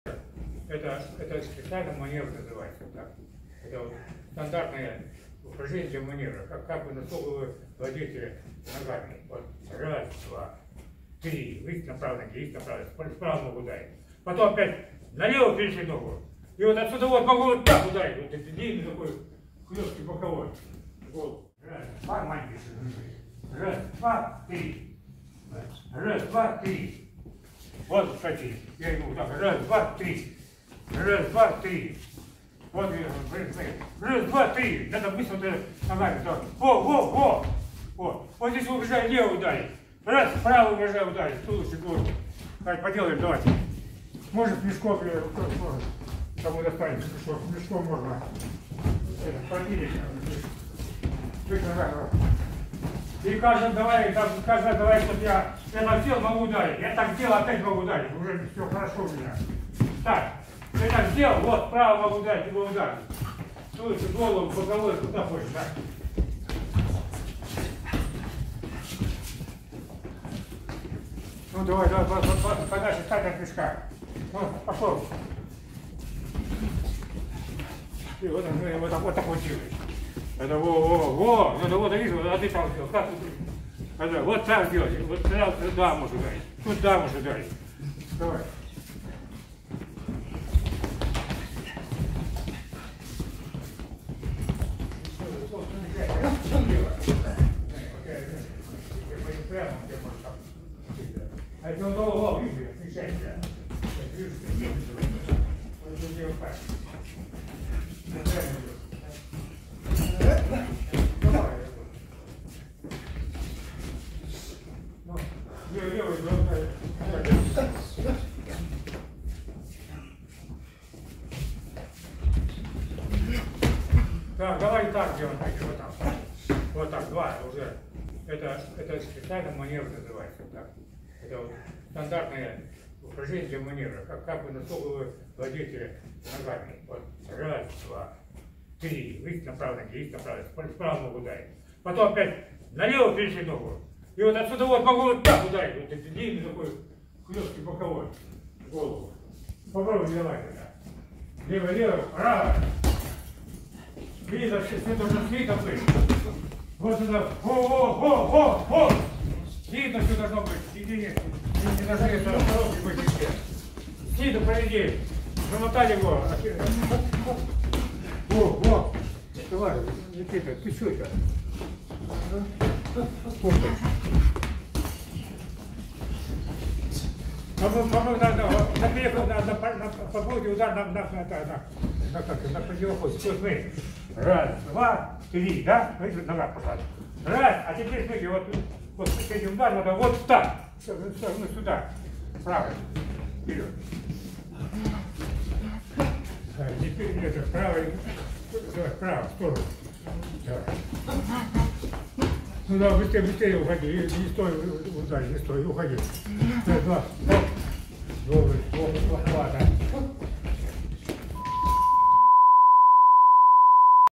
Бокс / Специальный маневр / Упражнение от мастера спорта СССР
Фрагмент тренировочного занятия по боксу
ДВГАФК
Зал Бокса
г. Хабаровск